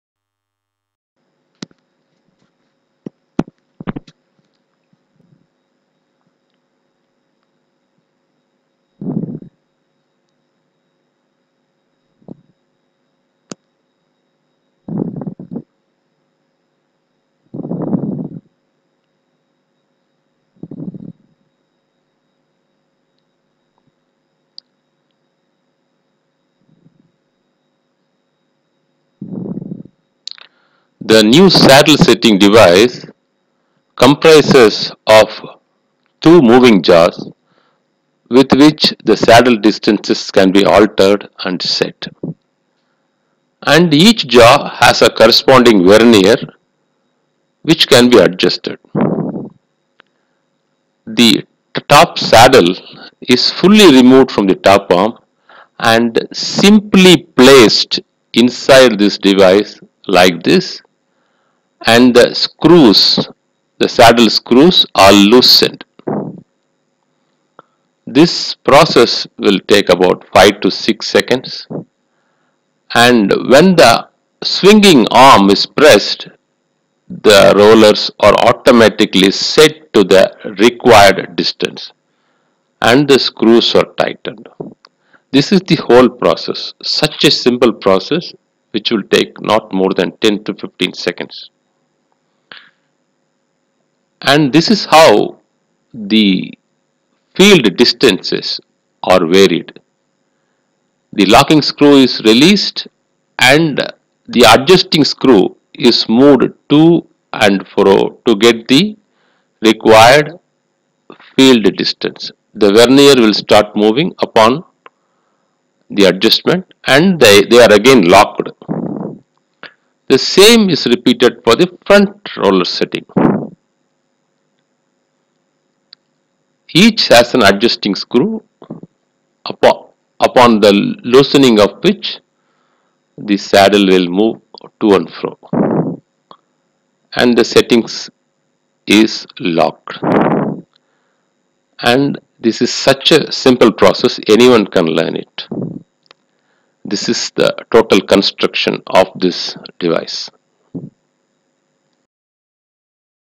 Apex Saddle Setting Device - New (2019)
For P 3-1 Top Arms, the saddle distances are easily set using this device.